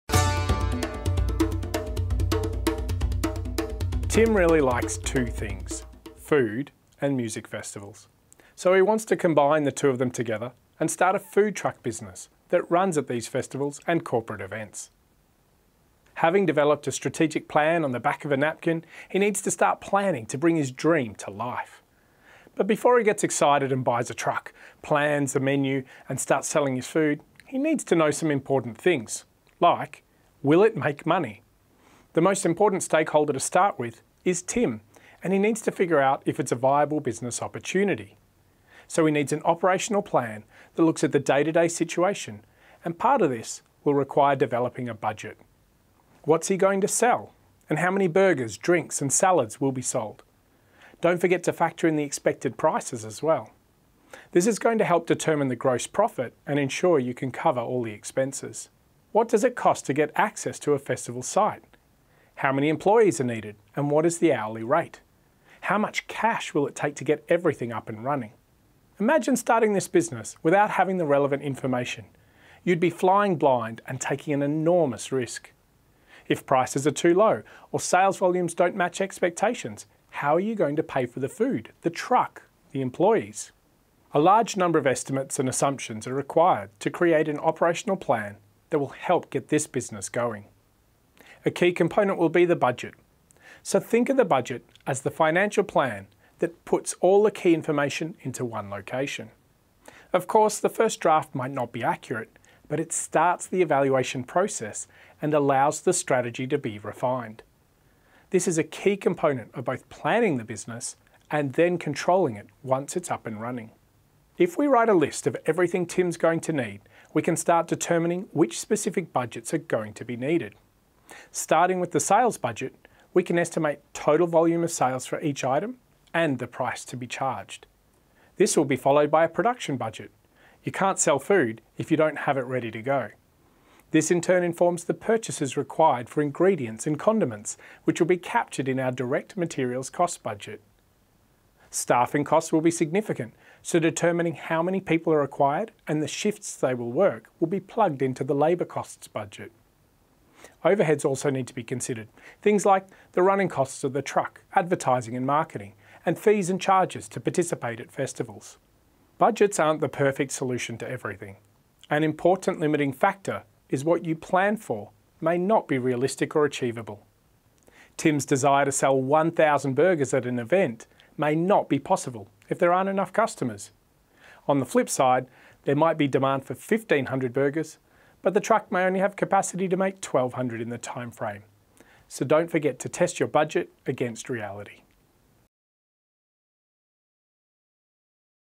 CPA Budgets
Tim wants to start a foodtruck business, and having a budget will help turn his strategy into a workable plan. This introduction to SMA Module 3 provides a simple example to highlight the link between strategy, operational plans and budgets.